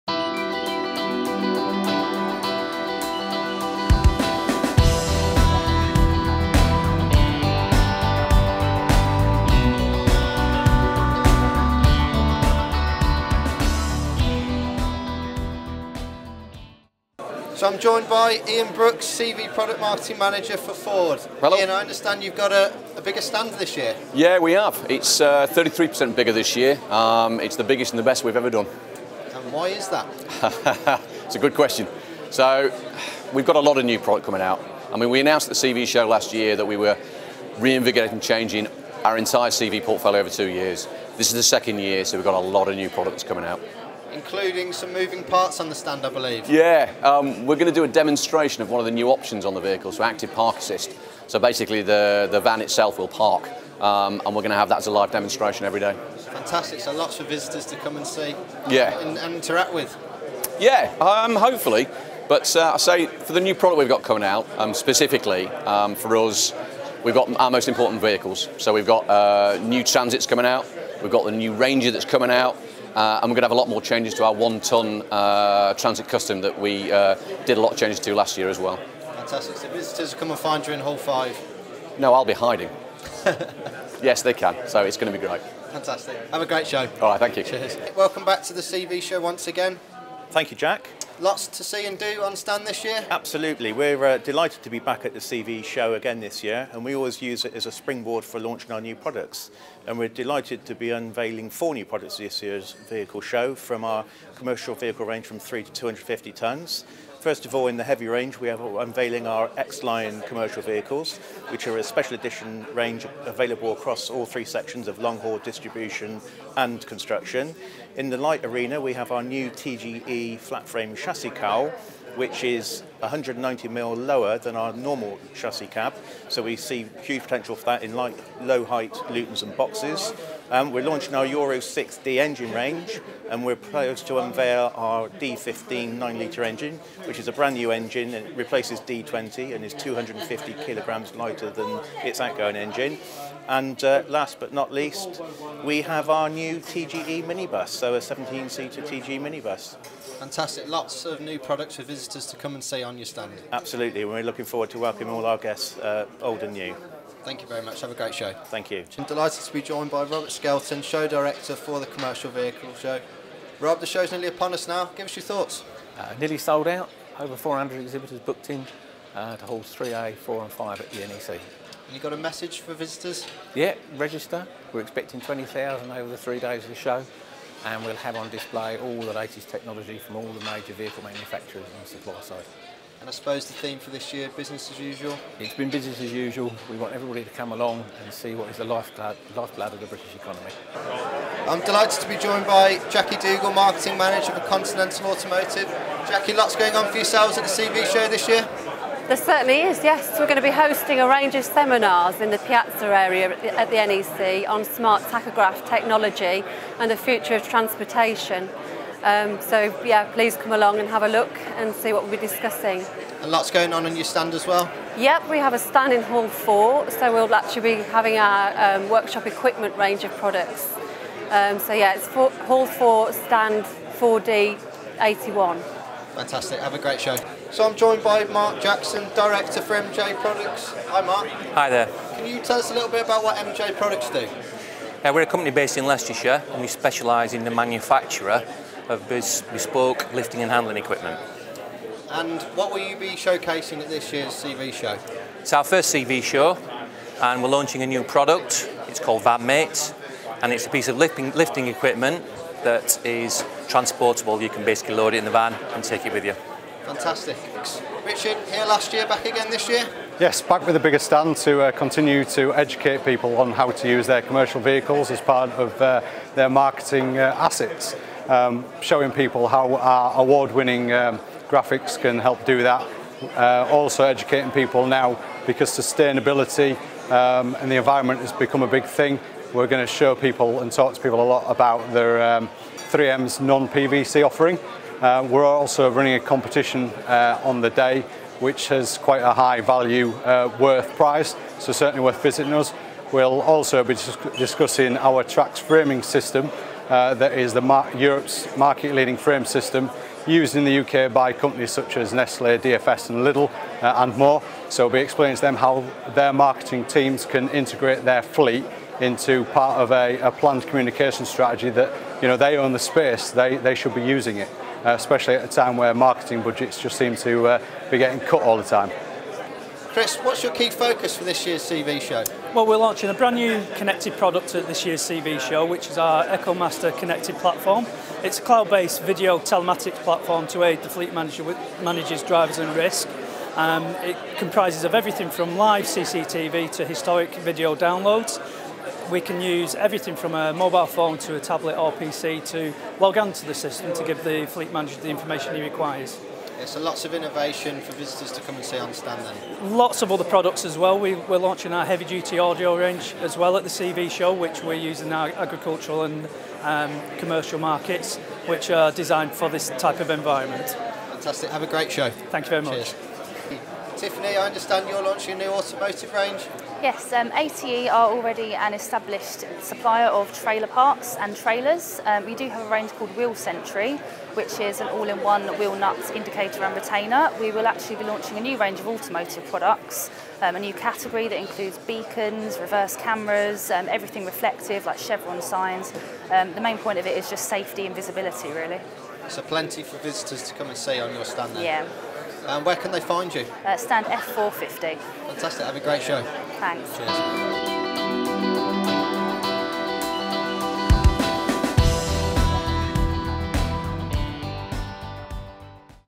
CV Show 2019 Media Day #1
On 28th February 2019 The CV Show held its annual Media Day at the NEC ahead of this year's show at the end of April. Exhibitors highlighted their products and spoke about the show in general. Video 1 of 4.